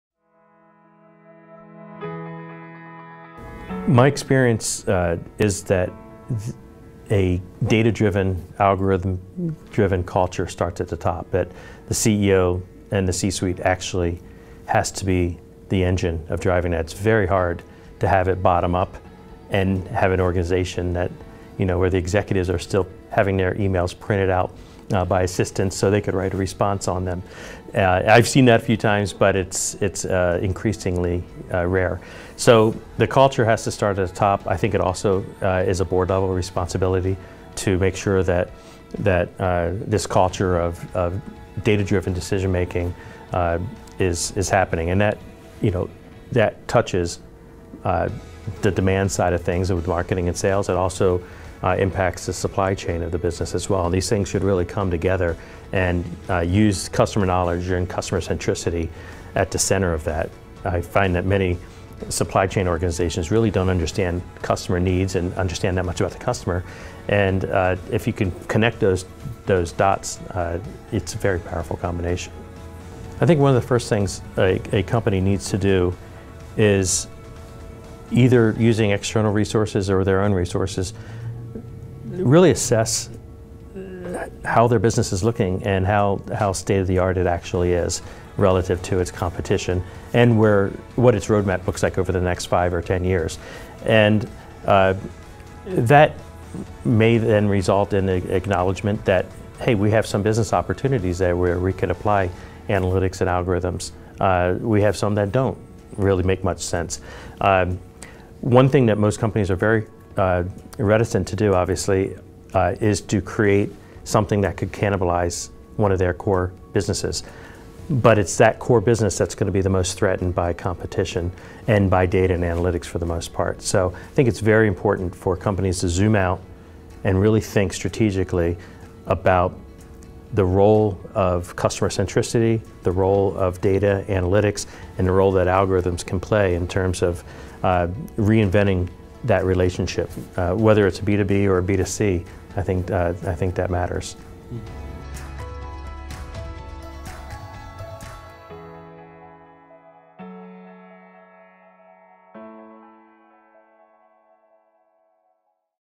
Algorithm And Data-Driven Culture In Business
Wes Nichols,  Venture Capitalist and Ex-CEO and co-founder of MarketShare shares his views on necessary data-driven culture and customer-centric supply chain in any business at the 8th Executive Leadership Forum at SMU Cox Business School in Dallas, Texas. Around 70 supply chain executives from various different industries attended the DSCI's eighth forum in October 2019.